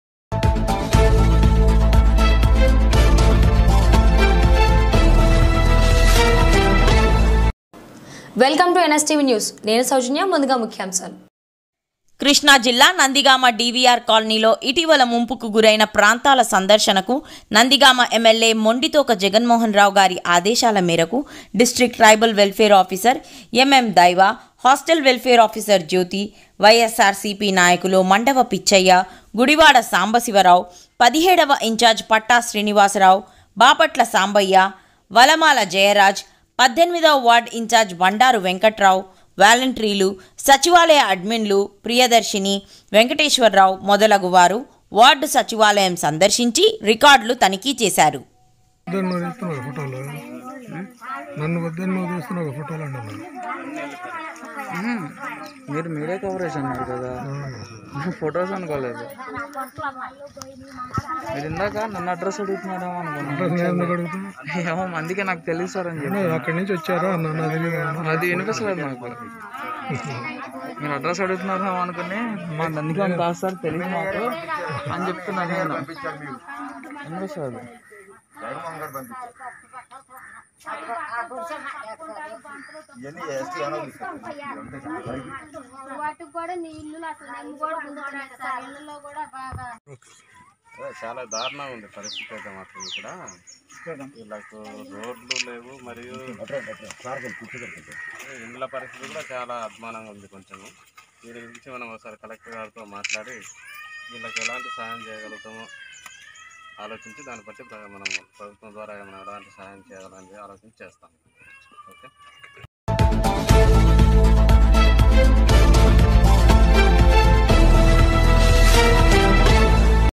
Nstv#నందిగామ DVR కాలనీలో ముంపు ప్రాంతాలను సందర్శించిన అధికారులు
Nstv#కృష్ణా జిల్లా,నందిగామ DVR కాలనీలో ముంపు ప్రాంతాలను సందర్శించిన అధికారులు